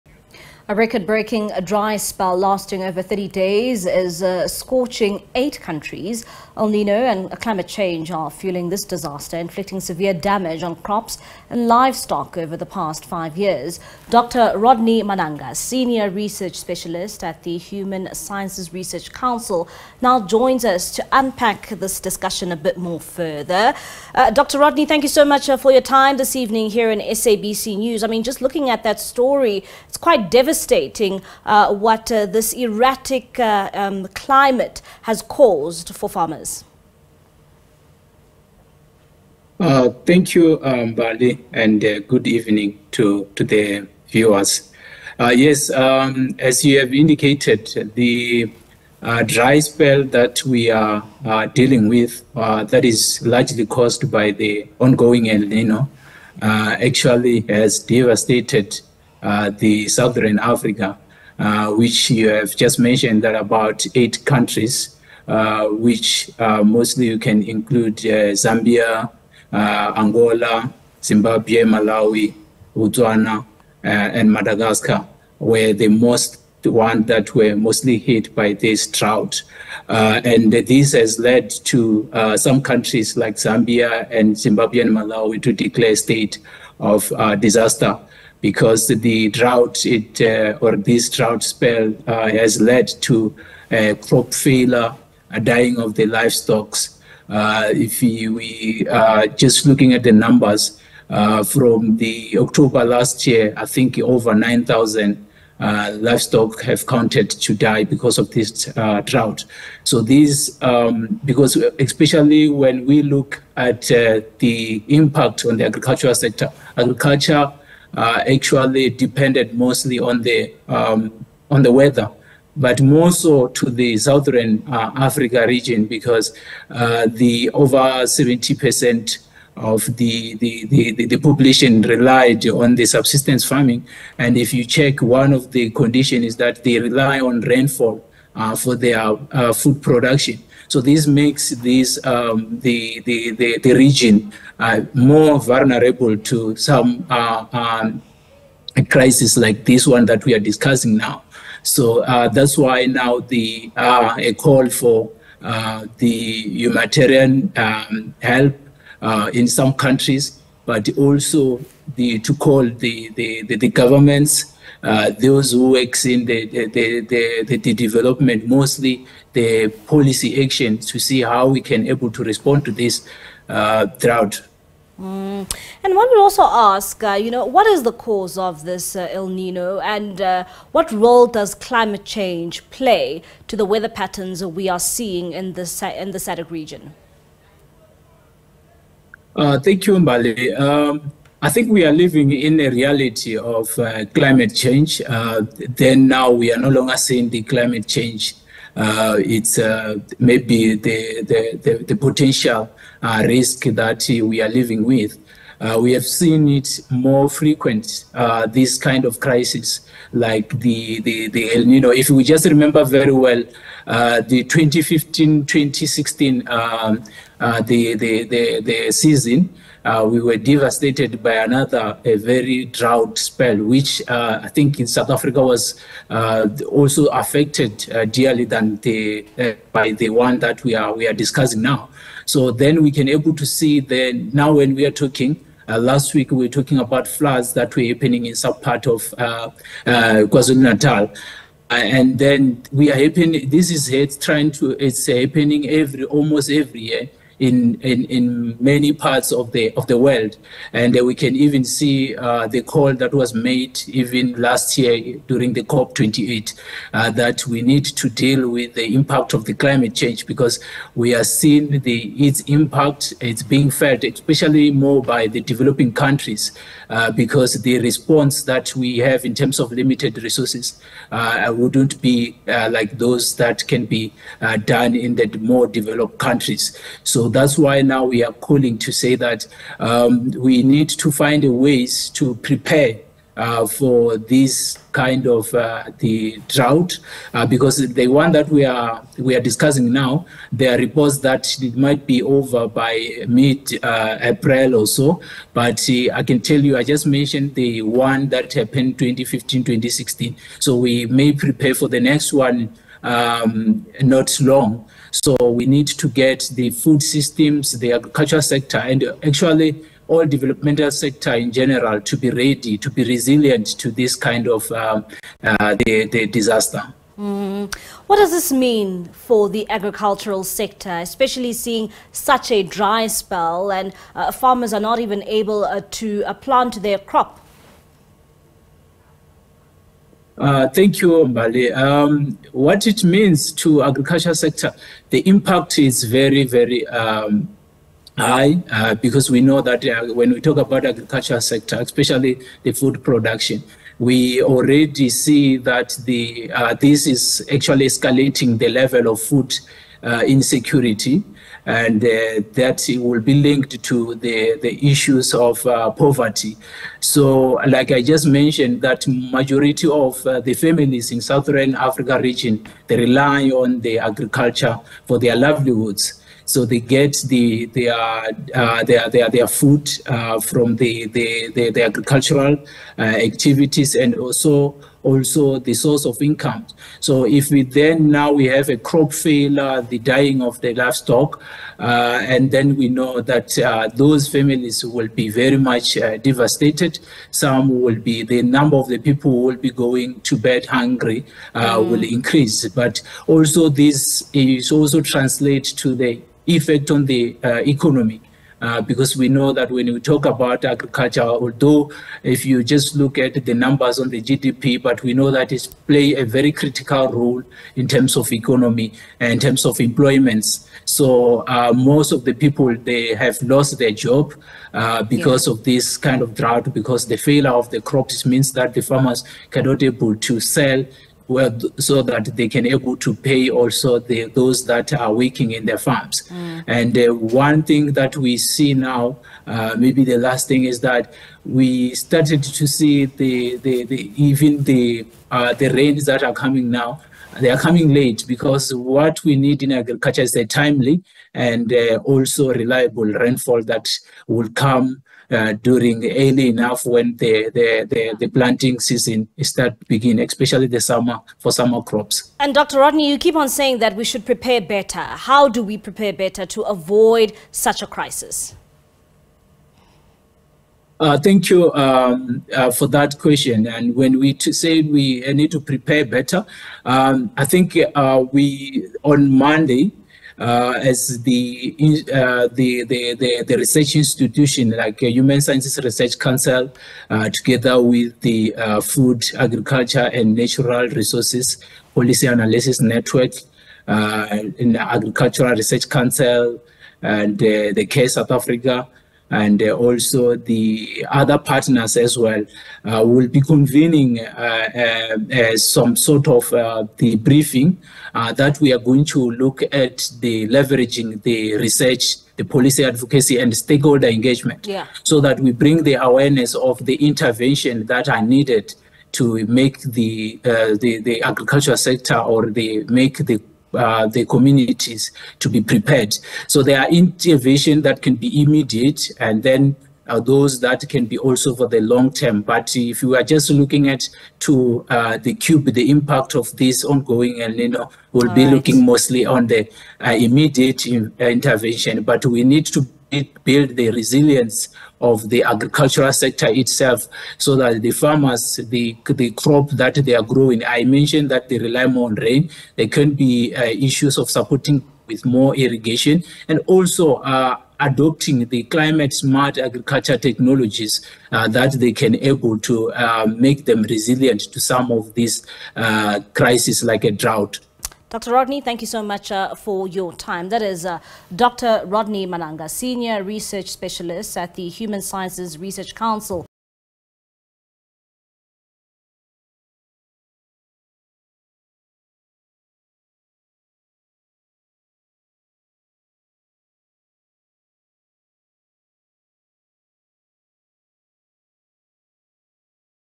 El Niño causes a dry spell: Dr Rodney Managa
A record-breaking dry spell lasting over 30 days is scorching eight countries.

El Niño and climate change are fueling this disaster, inflicting severe damage on crops and livestock over the past five years.

Dr Rodney Managa, Senior Research Specialist at the Human Sciences Research Council (HSRC) joins us to further the discussion.